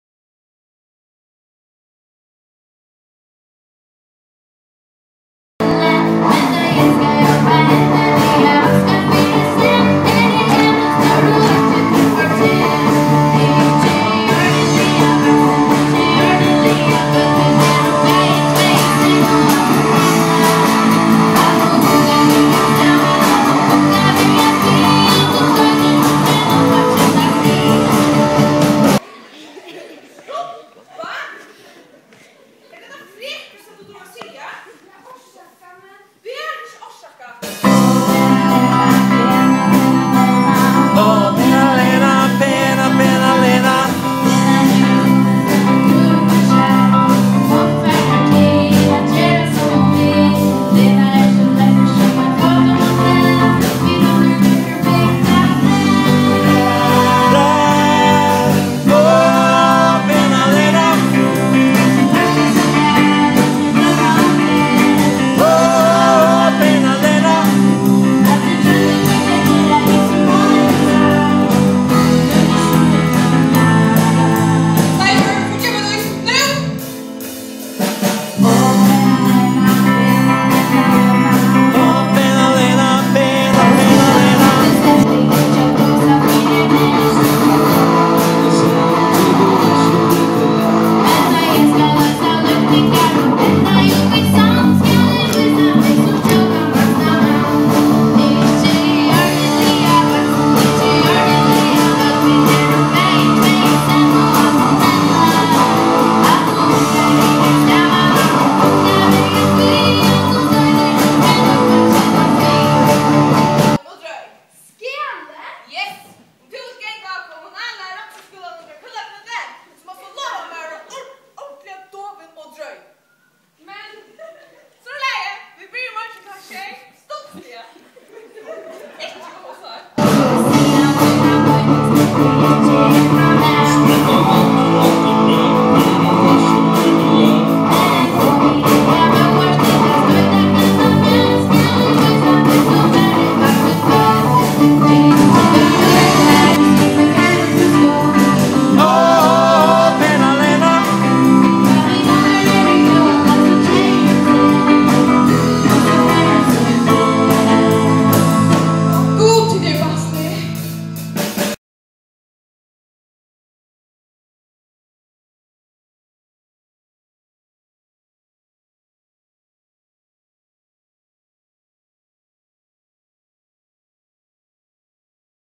Kúlla Knút
10. flokkur í skúlanum við Løkin hevur framført sjónleikin um Kúlla Knút. Her eru nakrar upptøkur tiknar frá orkestursetrunum (Ljóðið er ikki tað besta av tí sama).

90 minuttir við góðum sjónleiki, góðum tónleiki, góðum dansi og sera flottum leikpalli. Alt framleitt og framført av næmingunum!